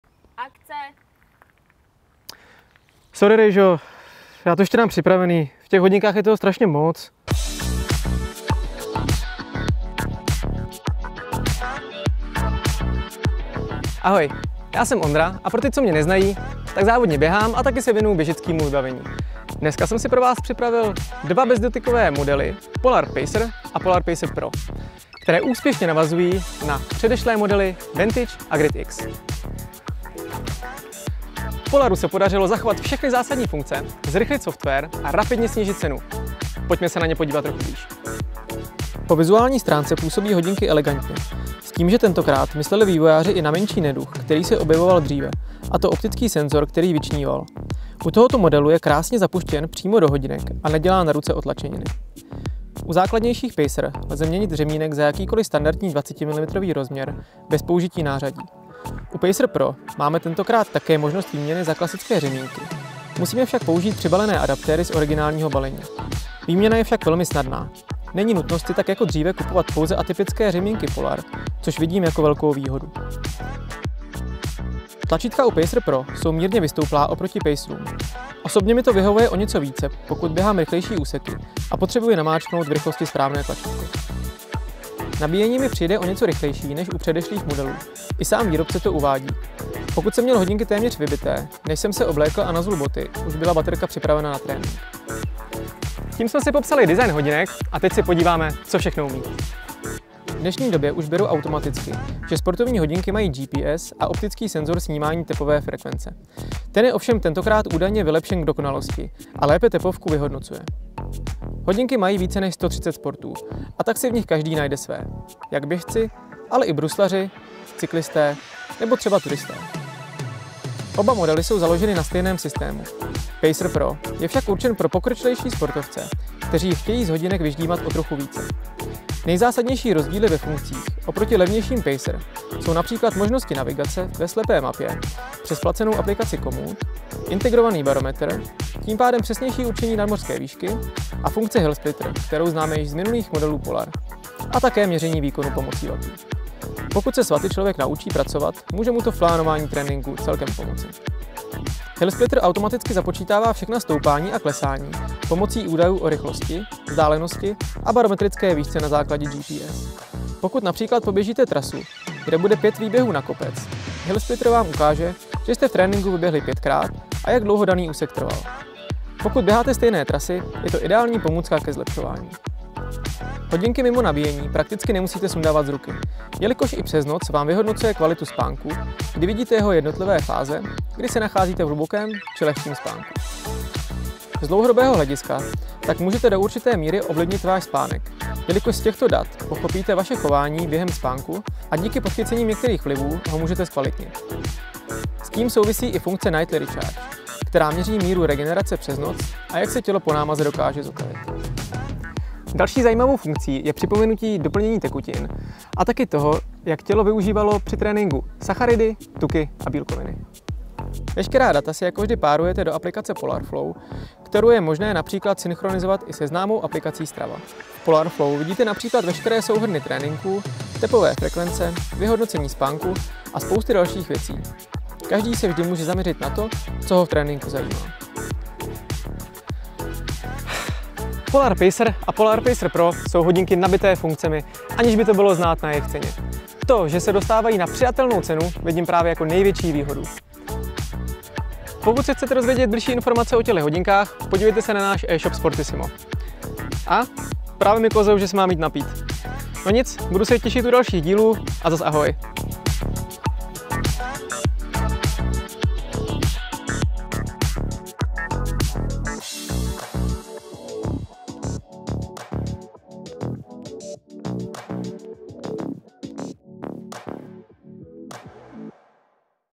RECENZE | Hodinky Polar Pacer a Pacer Pro
⌚🏃 Recenze sportovcům ušitá na míru! Zkoukněte novinky od Polaru, v tomto případě tu pro vás máme multisportovní hodinky Polar Pacer a Polar Pacer Pro. Vylepšený design, přidané funkce a hlavně zajímavá cena. No přesvědčte se sami. 😉